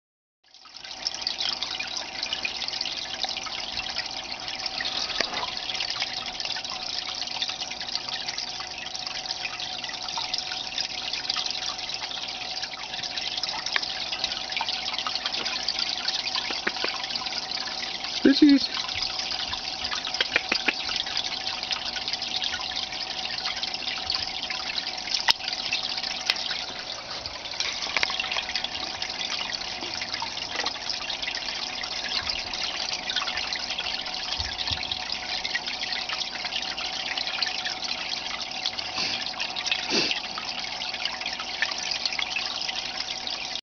Oscar Cichlid - Circus Training
Training my Oscars to jump from the tank with the eventual goal of jumping through hoops.  This eventually backfired when one of them jumped from the tank when I opened it for feeding.  This video is like 5 years old.